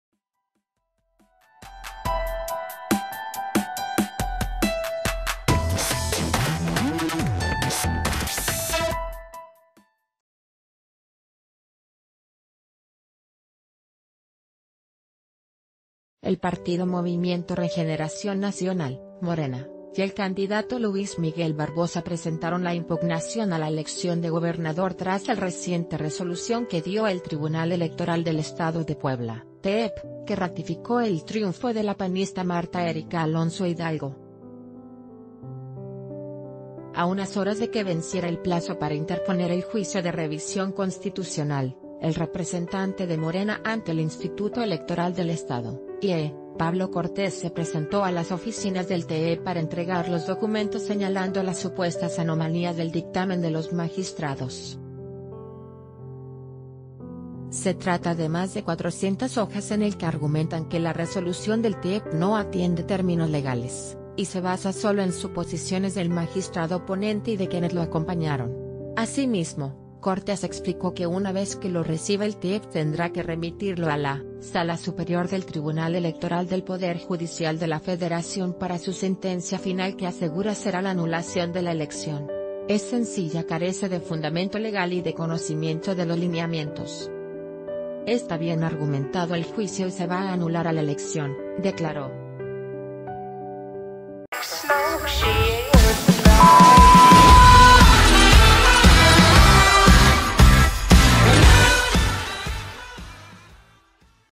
Presentan Barbosa impugnación de la elección a gobernador
El partido Movimiento Regeneración Nacional (Morena) y el candidato Luis Miguel Barbosa presentaron la impugnación a la elección de gobernador tras el reciente resolución que dio el Tribunal Electoral del Estado de Puebla (TEEP) que ratificó el triunfo de la panista Martha Erika Alonso Hidalgo. A un...